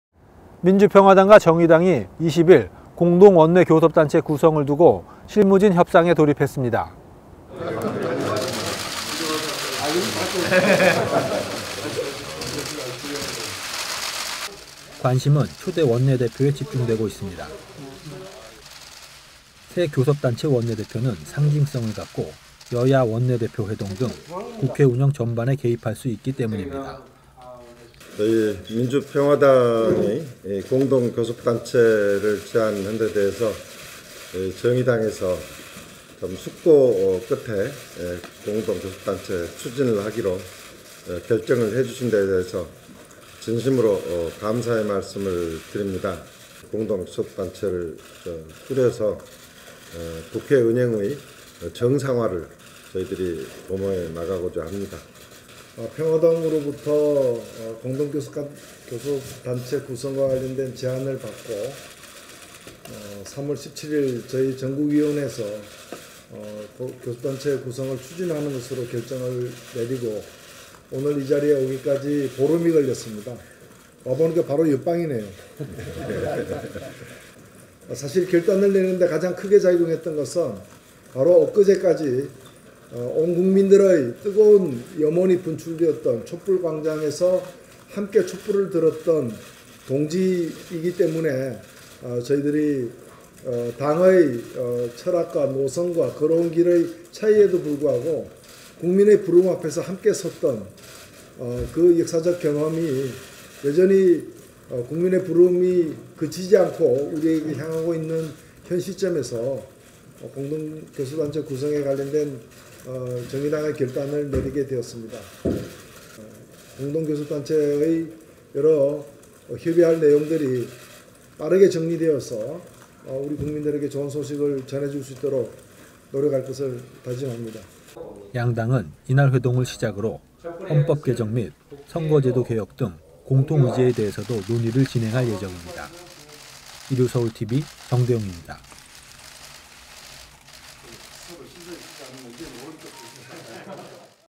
민주평화당-정의당, 공동 원내교섭단체 구성 실무협상
장병완 민주평화당 원내대표와 노회찬 정의당 원내대표가 20일 국회에서 공동 원내교섭단체 구성 논의를 위해 회동하고 있다.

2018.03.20 일요서울TV 정대웅 기자